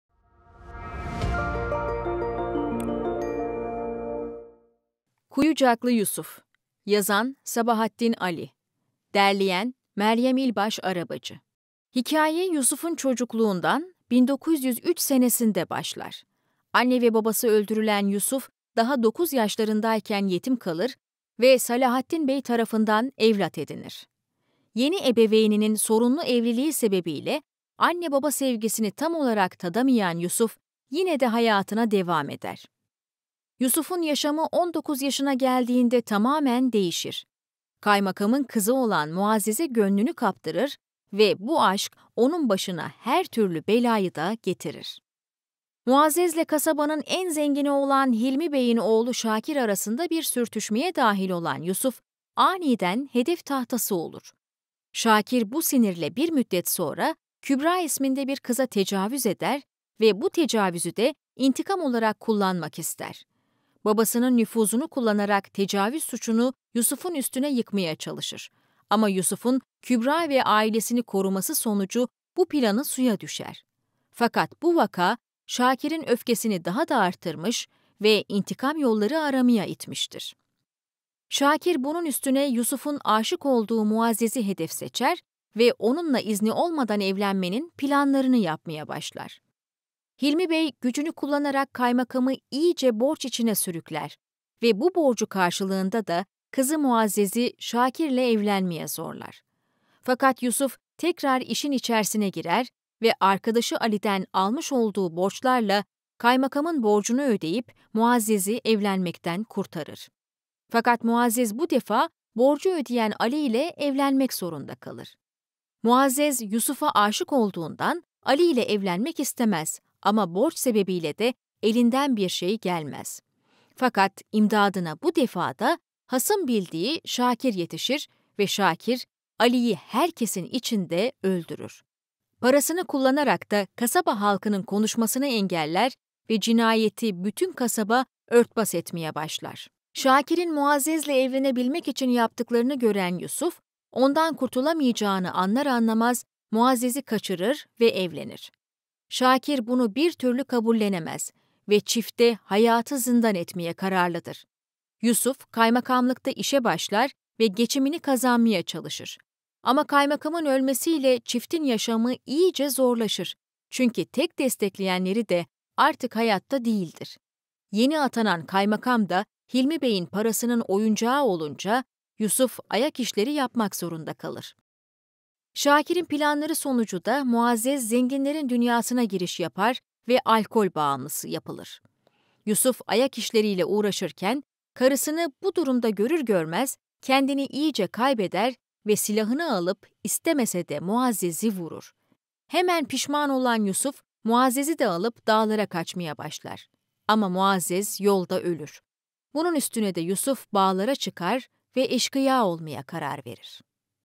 KUYUCAKLI YUSUF SESLİ KİTAP ÖZETİ - Sabahattin Ali - Özet Kitap - Kitap Özeti Dinle.
Sabahattin Ali'nin en çok okunan kitaplarından Kuyucaklı Yusuf sesli kitap özeti yayında.

Sabahattin Ali'nin kaleme aldığı artık klasikleşmiş bir Türk romanı olan Kuyucaklı Yusuf sesli kitap özetini şimdi dinleyebilirsiniz.

İyi seyirler.